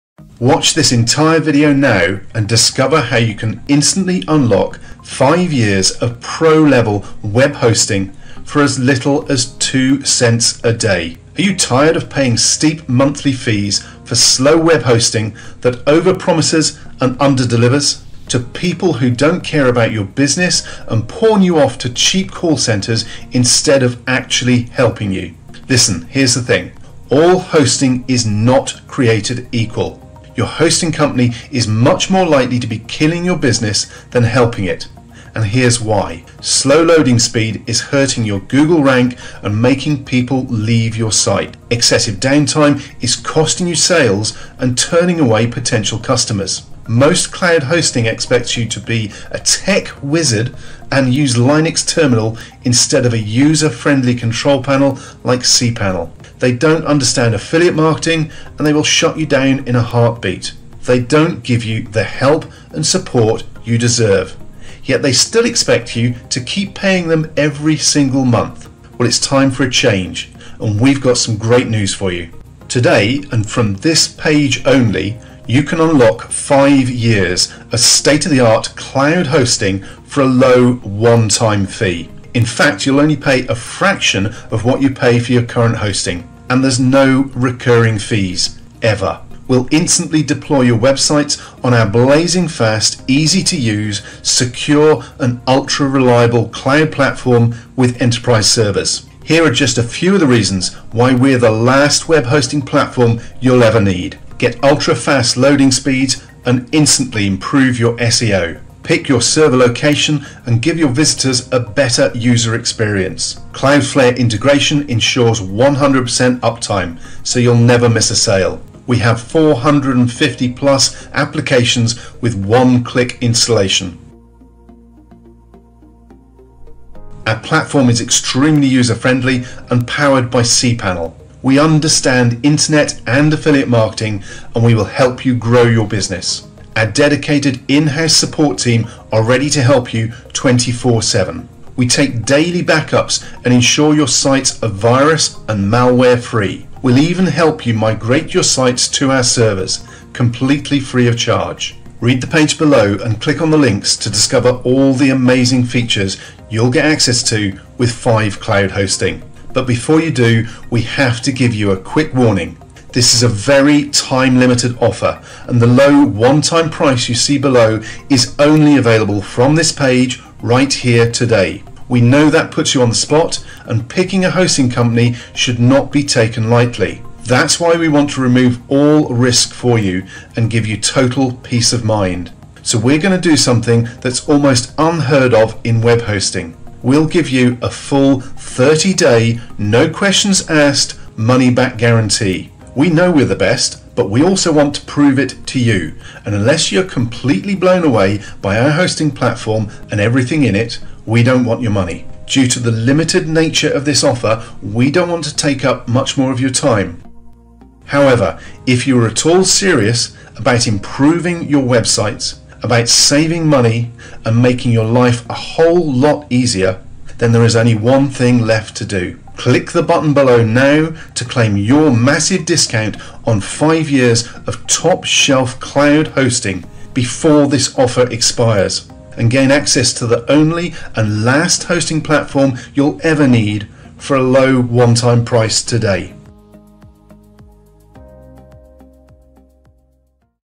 5CLOUDHOST Review Budget Friendly Web Hosting LET ME SHOW YOU WHAT IT IS
5cloud hosting platform is naturally powered by cPanel. It's the most popular web-based control panel for a reason. It’s extremely user friendly and helps you easily manage your websites with automation tools designed to simplify everything.